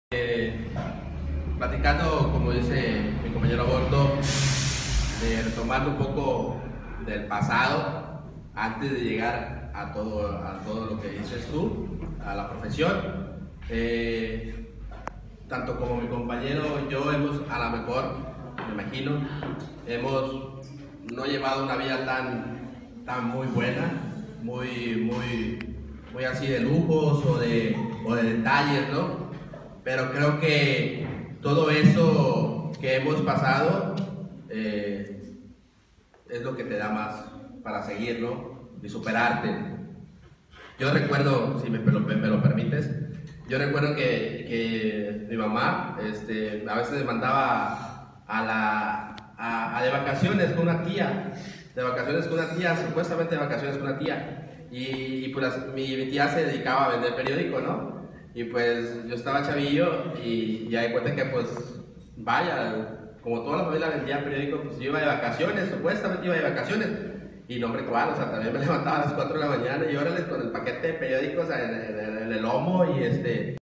Las "vacaciones" de Lumino
LUMINO nos habla de las vacaciones que disfrutaba con su tía, todo esto con motivo de la charla llevada acabo en la Preparatoria Saint Germain en Tampico, Tamaulipas.